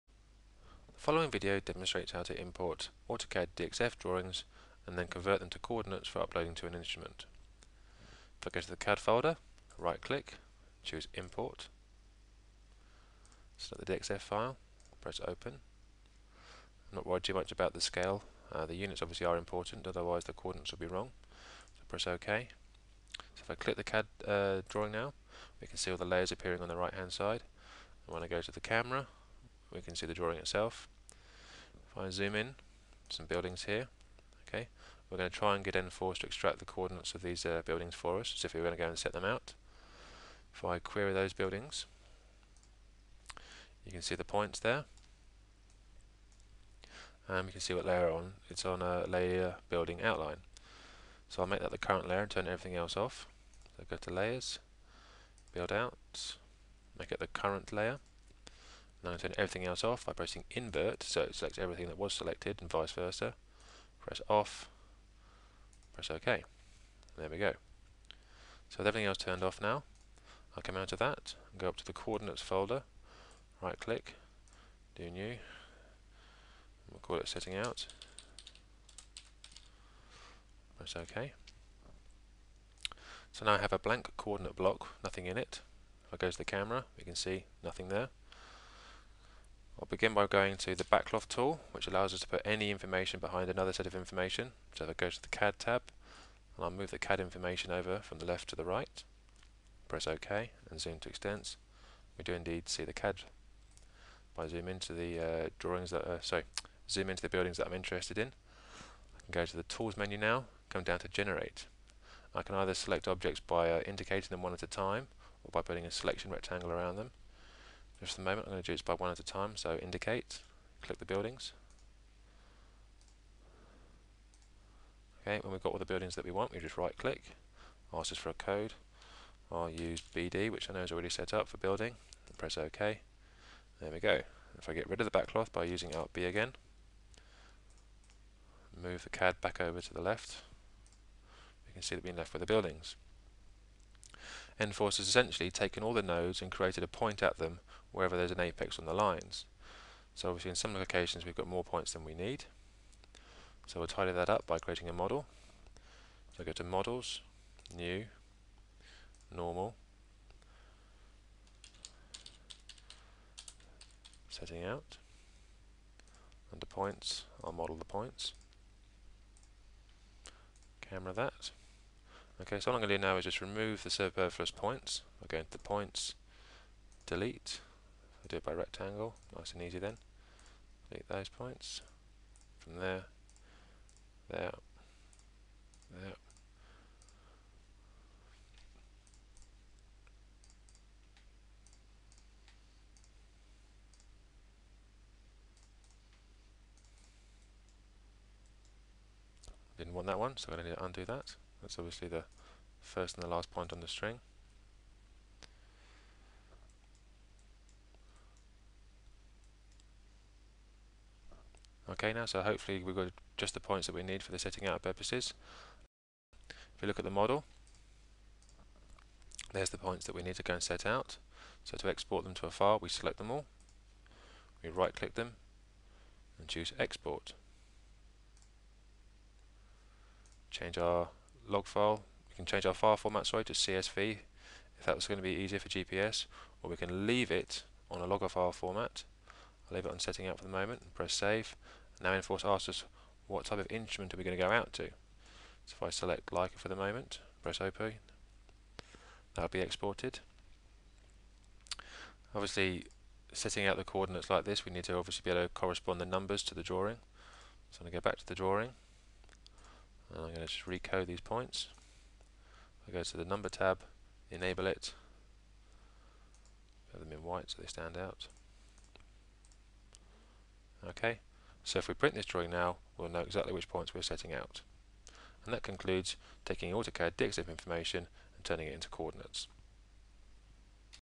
Extracting Setting Out Data From a DXF File With n4ce
This video demonstrates how to quickly import DXF data and extract the coordinates of objects for setting out purposes.